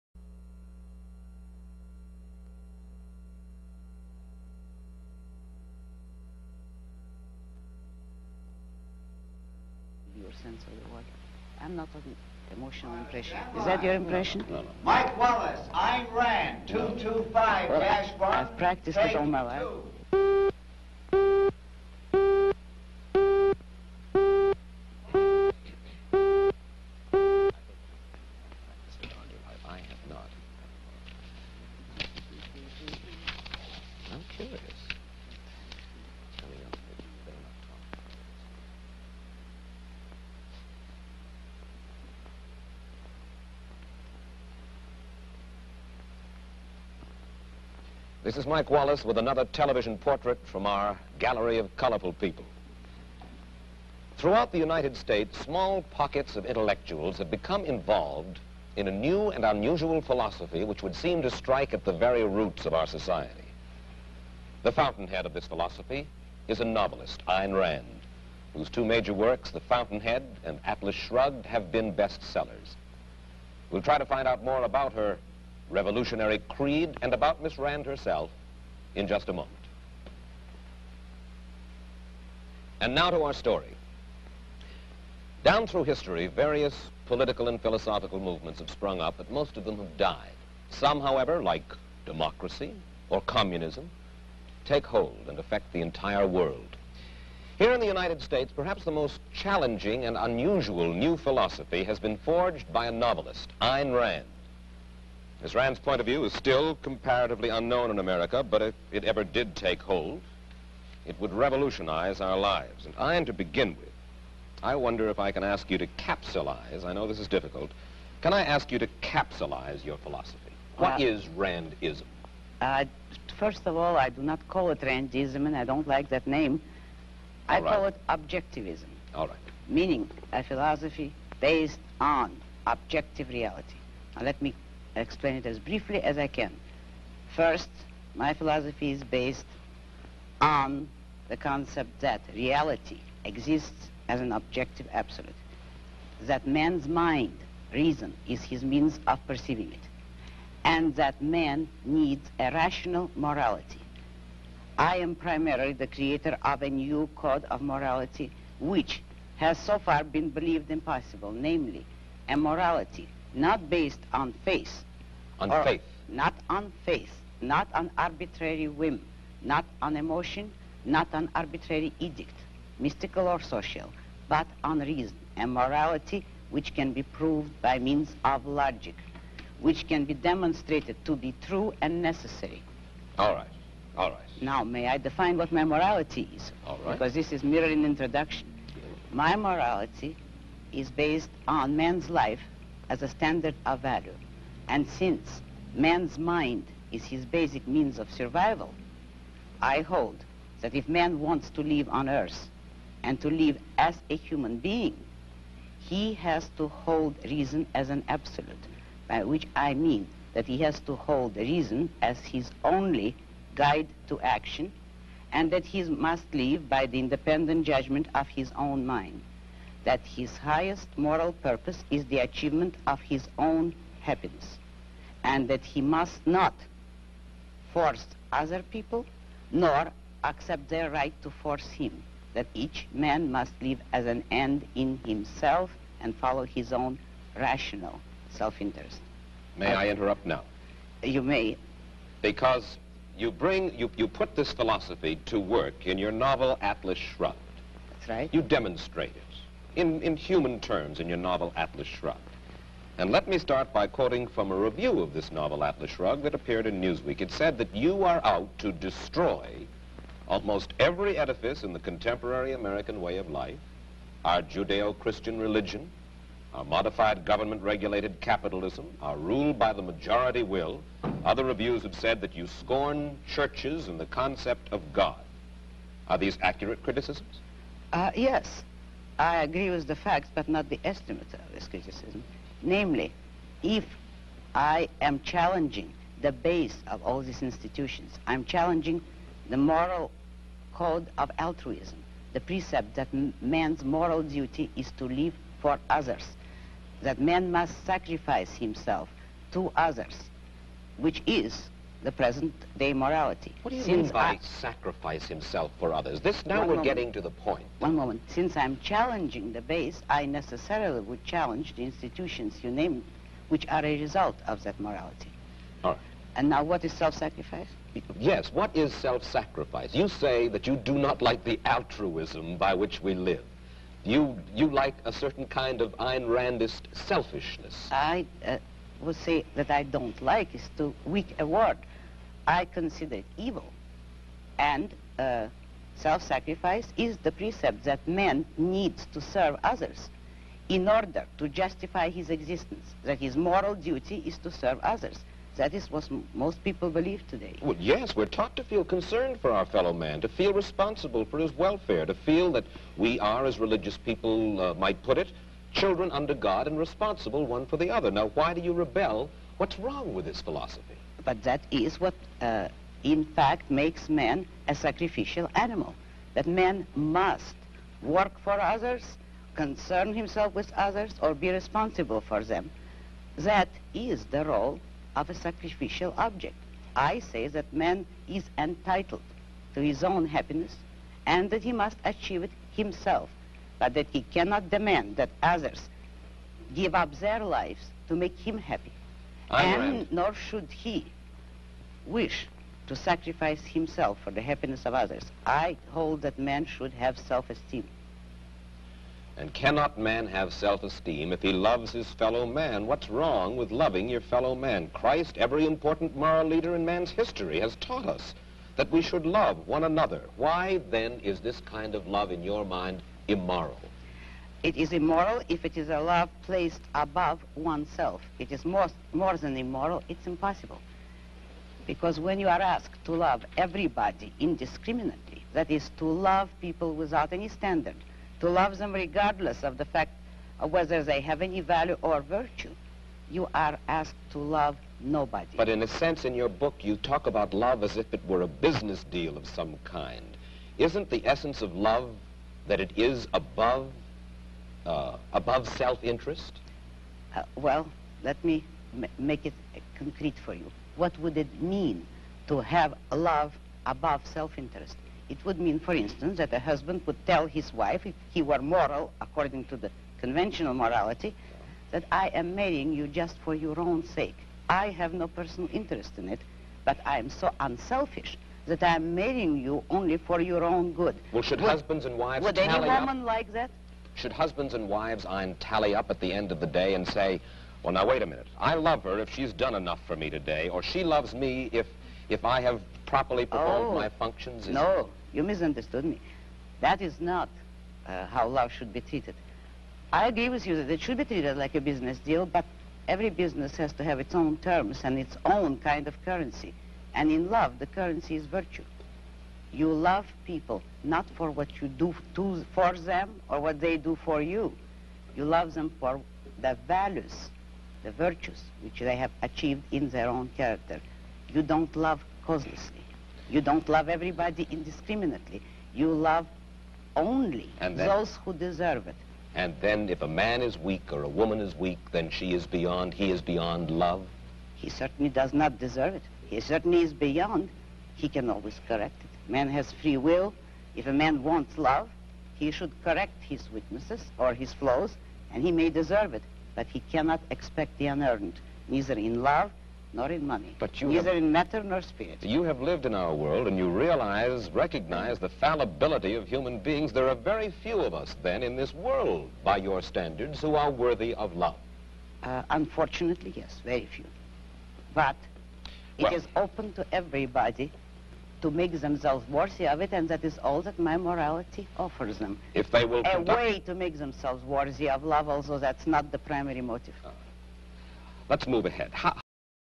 Ayn Rand interviewed by Mike Wallace about Altruism 1959 *Part 1*
Ayn Rand attempts to explain her philosophy based on objectivism to the more conservative Wallace, clearly she was way ahead of her time & her books The Fountainhead & Atlas Shrugged are some of the most popular books in the history of publishing...
Ayn Rand believed in redefining altruism, making man responsible for his own happiness first and foremost, Wallace tries to hang on, but you can tell after only 1 minute it turns into a showdown -- I love me some Ayn Rand, don't you??